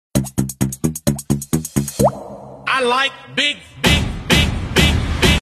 I Like The Big Berkey and I Cannot Lie!
Finding yourself overwhelmed and confused, not sure what water filter system is best or what filter to go with to get clean drinking water in your home?🤷‍♀️

My wife and I felt the same way back in 2006.

We tried a Brita at first but then became quickly dissatisfied because it only filtered 7 contaminants and there were way more in the public water supply.🤢

Then we discovered something BIG!

That’s right!  The BIG BERKEY!🤩

The Big Berkey is the most famous gravity water filtration system in the world!

When I say I like the Big Berkey I mean people across the world LOVE the Big Berkey.😍

If you’re thinking about trying the most powerful gravity water filtration system in the world I invite you to click the link in my bio and learn more about which Berkey water purification system might be right for you!

-Jeff